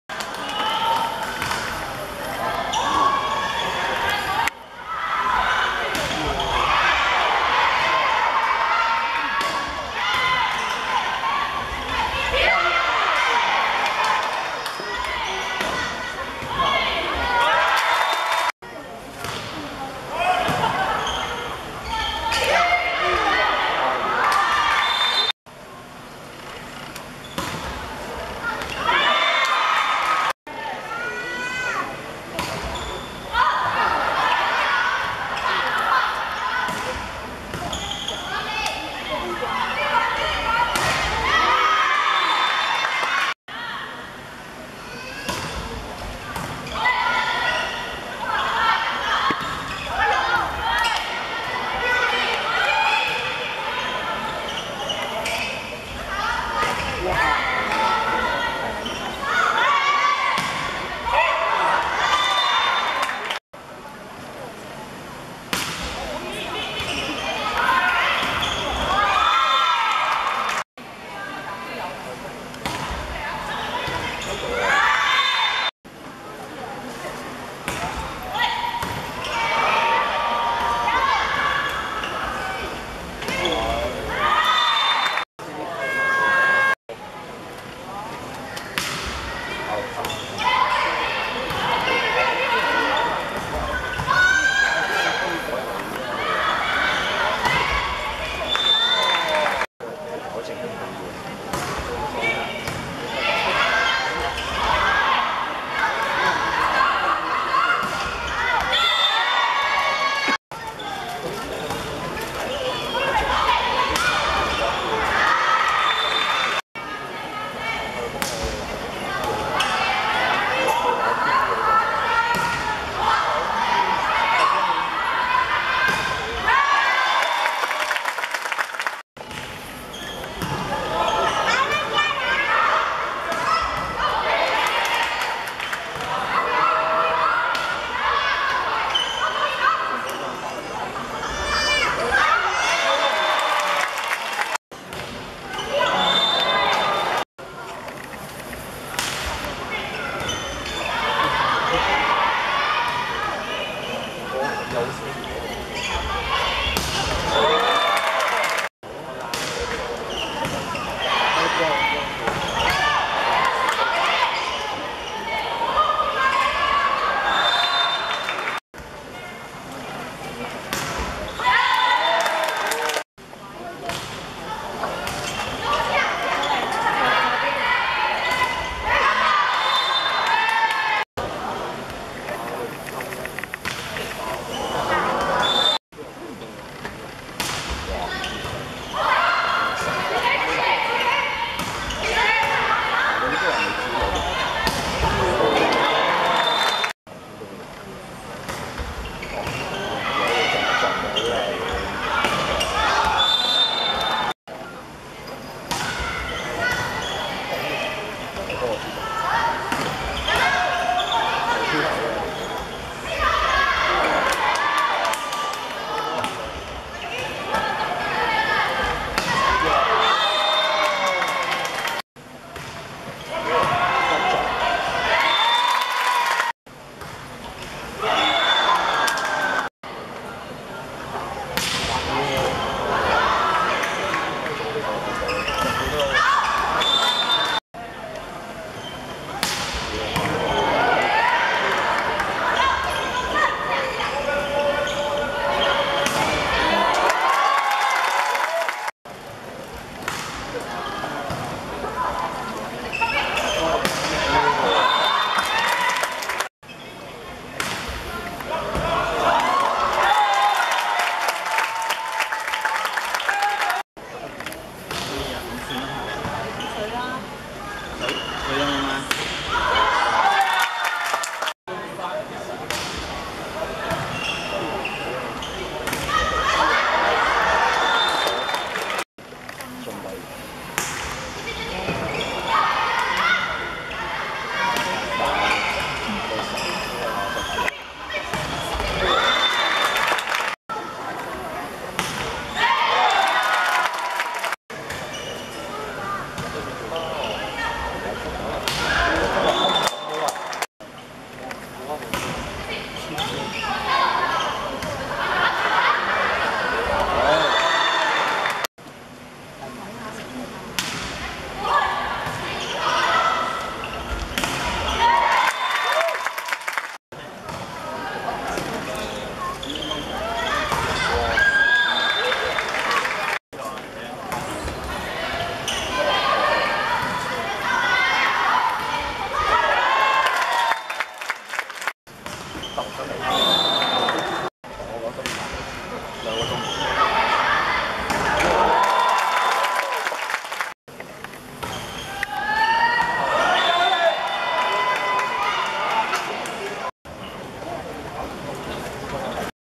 中華台北vs香港(亞洲東區女子排球錦標賽2014)第一局精華
2014.7.4.馬鞍山體育館 19:30 中華台北(白) 25:16 香港(紅)